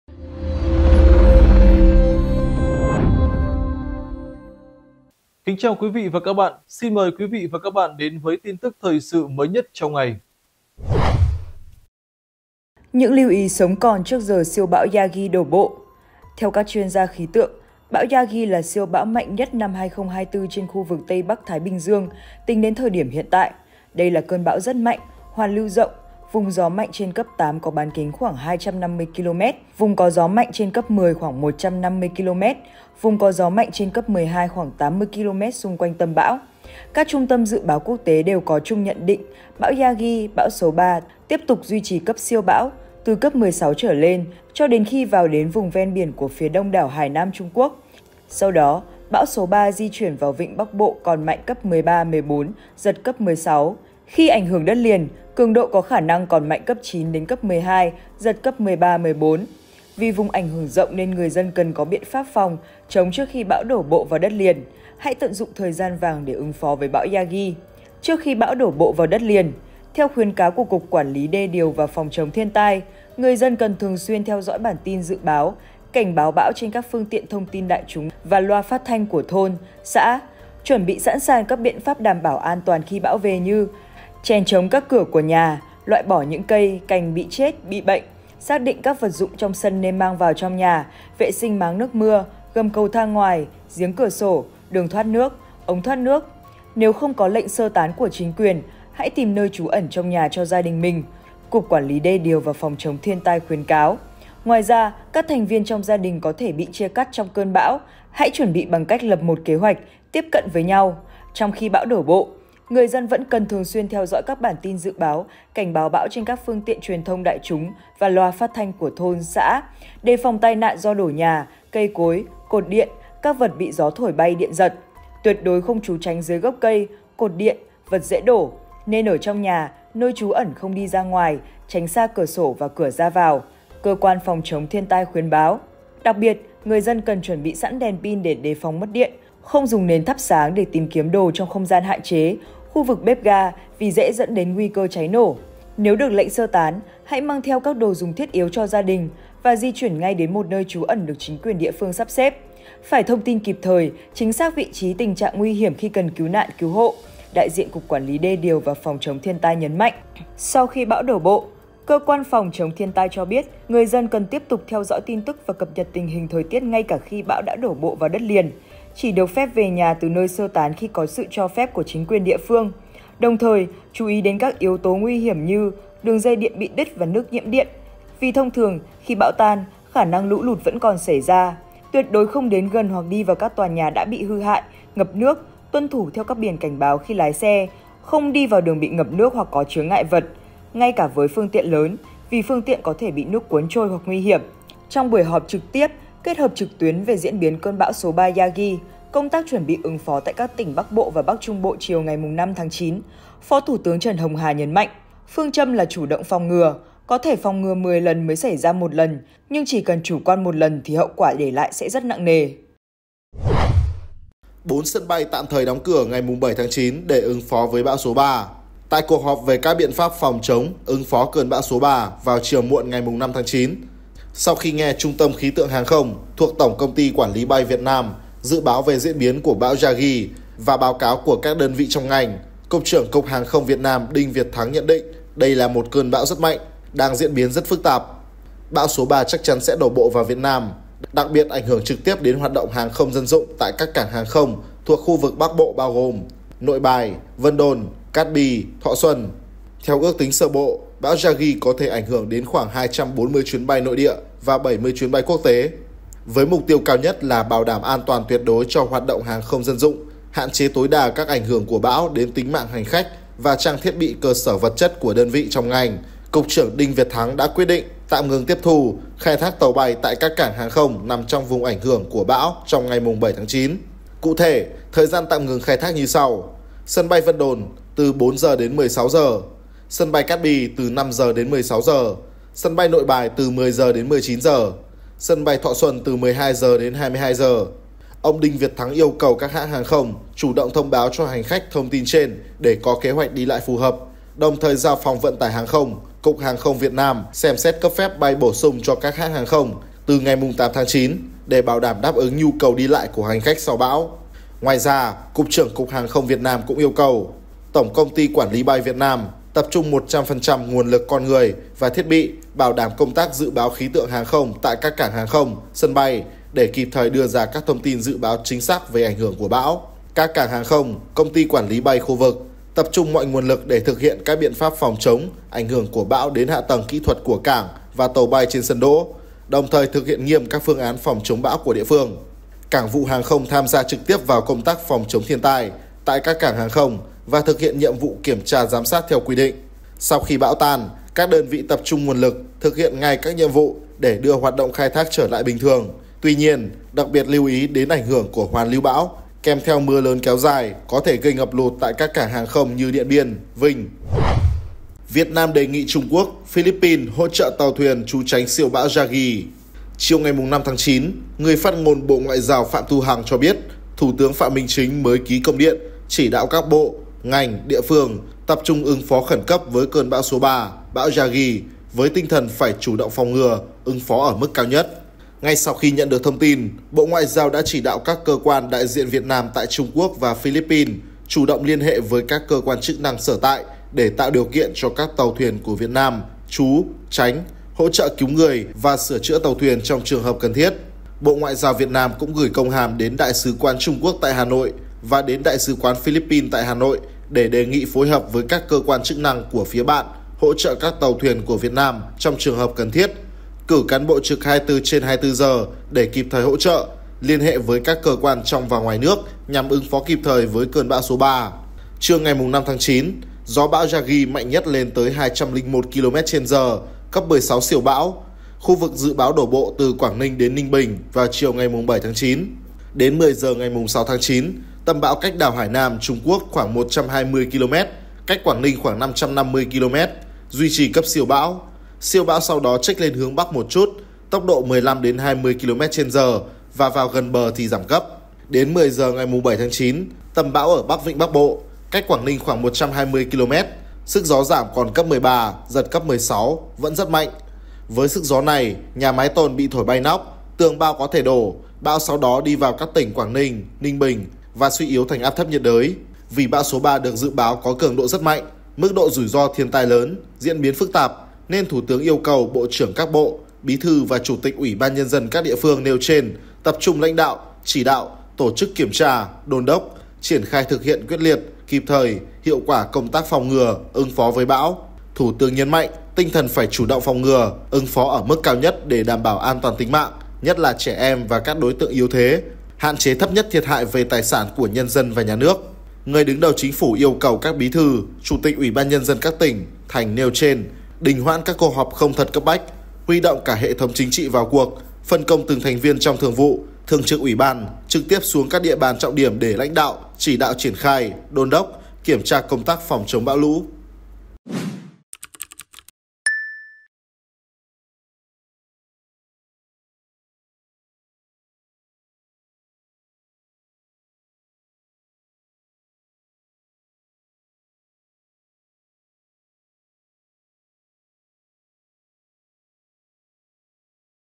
Những lưu ý sống còn trước giờ siêu bão số 3 Yagi đổ bộ | Báo VietNamNet
Siêu bão Yagi được đánh giá là bão mạnh nhất năm 2024 trên khu vực Tây Bắc Thái Bình Dương. Vì vậy, người dân cần thực hiện các biện pháp phòng chống kịp thời trước giờ bão đổ bộ.